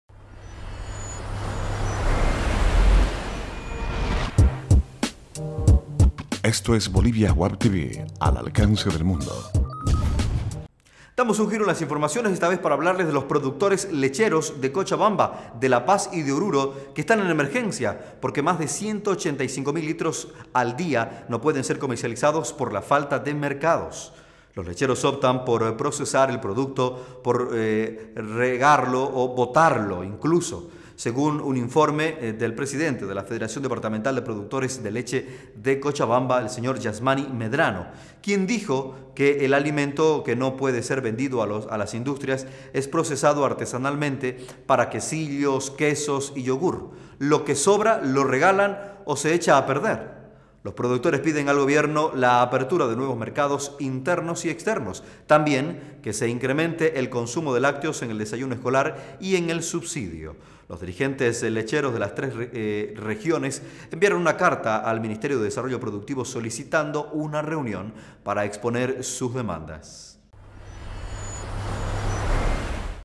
Bolivia News - Noticia del dia 8 octubre 2015
Los productores lecheros de Cochabamba, La Paz y Oruro están en emergencia porque más de 185 mil litros al día no pueden ser comercializados por falta de mercados. Los lecheros optan por procesar el producto, por regalarlo o botarlo. Según informó el presidente de la Federación Departamental de Productores de Leche de Cochabamba, Yhasmany Medrano, quien dijo que el alimento que no puede ser  vendido a las industrias es procesado artesanalmente para quesillos, quesos y yogur. Lo que sobra lo regalan o se echa a perder. Los productores piden al Gobierno la apertura de nuevos mercados internos y externos; también, que se incremente el consumo de  lácteos en el desayuno escolar y en el subsidio. Los dirigentes lecheros de las tres regiones enviaron una carta al Ministerio de Desarrollo Productivo, solicitando una reunión para exponer sus demandas.